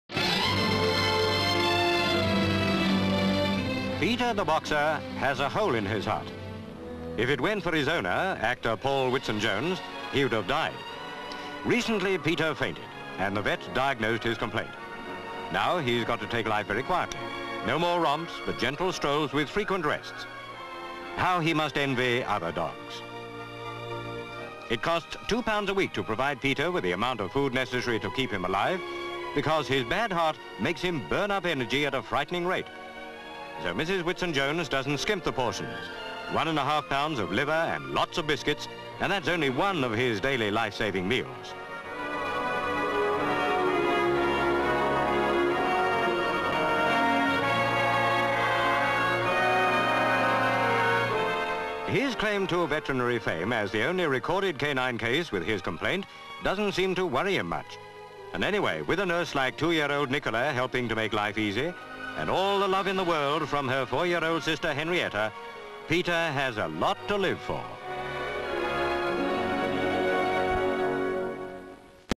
Dog Has Hole In Heart (1966)
Hampstead, London.

SV. Pan, Paul Whitsun-Jones walking his boxer dog Peter who has a hole in his heart. SV. He comes towards camera, stops and Peter sits. CU. Peter lays down looking around. SV. Pan, Alsatian runs through the park. LV. Two other dogs running in park, Peter cannot run any more. LV. Paul has to drag Peter along as he tries to get away and play with other dogs. LV. Paul walks into home with dog. SV. Mrs Jones preparing the dog's meal. CU. Mrs Jones. CU. As she prepares the meal. CU. Peter watching, licking his lips. LV. Mrs Jones puts his dinner down and pulls him over. CU. As the dog eats. GV. Peter climbs into his basket for a rest. CU. Two year old Nichola puts the dog's cowl into his basket for him. CU. Pan as four year old Henrietta cuddles Peter while he sits in basket.
 FILM ID:1783.26